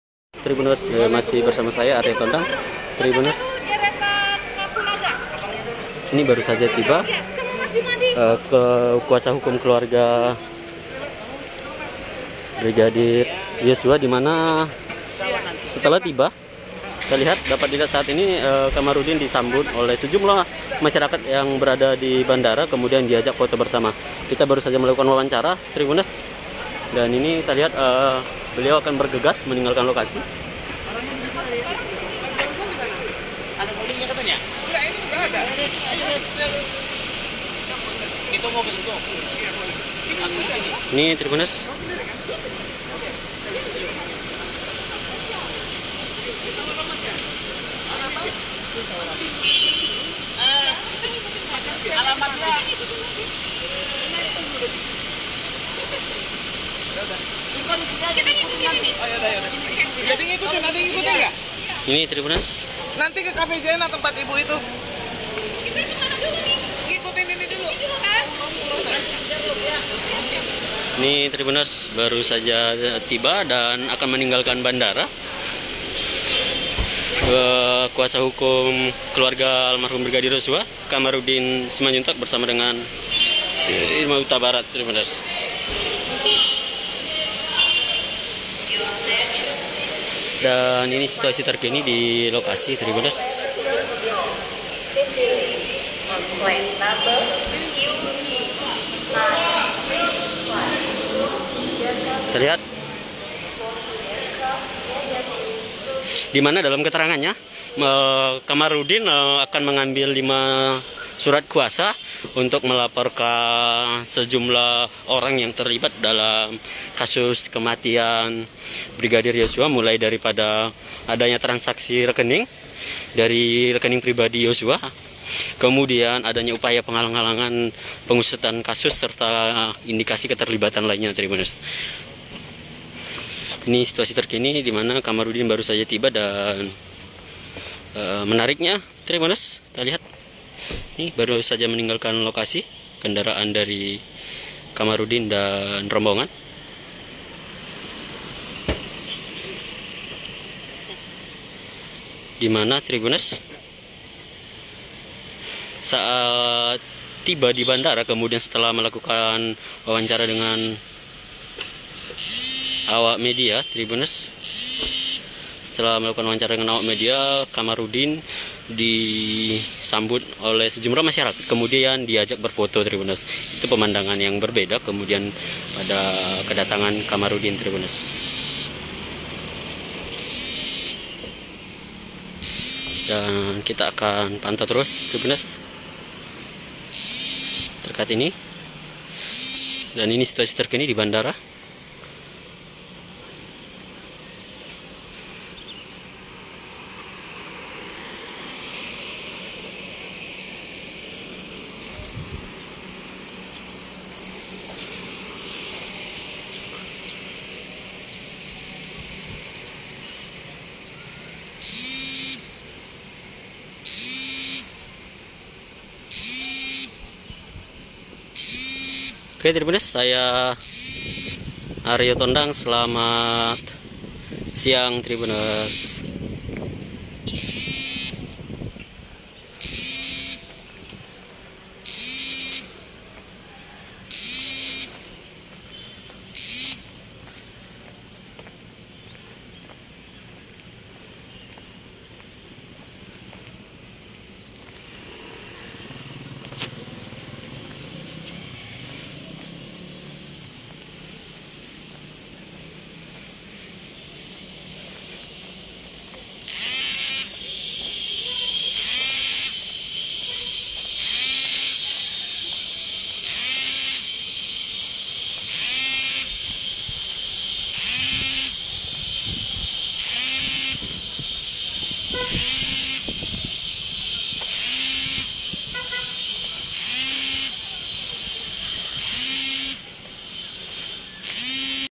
Tiba di Jambi, Pengacara Brigadir Yosua Diajak Foto oleh Masyarakat
Tiba di Jambi, Pengacara Brigadir Yosua Diajak Foto oleh Masyarakat (waw)

Kuasa Hukum keluarga Almarhum Brigadir Nofriansyah Yosua Hutabarat alias Brigadir Yosua, Kamaruddin Simanjuntak dan Irma Hutabarat tiba di Jambi, Kamis (18/8/2022).

Kamarudin, dan Irma tiba di Bandara Sultan Thaha Jambi pada pukul 13:00 WIB.

Kamarudin menjelaskan, kedatangnyannya ke Jambi untuk mengambil atau meminta tanda tangan surat kuasa.

Tidak tanggung-tanggung, Kamaruddin menjelaskan, ia akan meminta 5 surat kuasa sekaligus, yakni melaporkan Irjen Pol Ferdy Sambo dan istrinya Putri Chandrawati yang membuat laporan palsu terkait tuduhan Brigadir Yosua melakukan pelecehan seksual.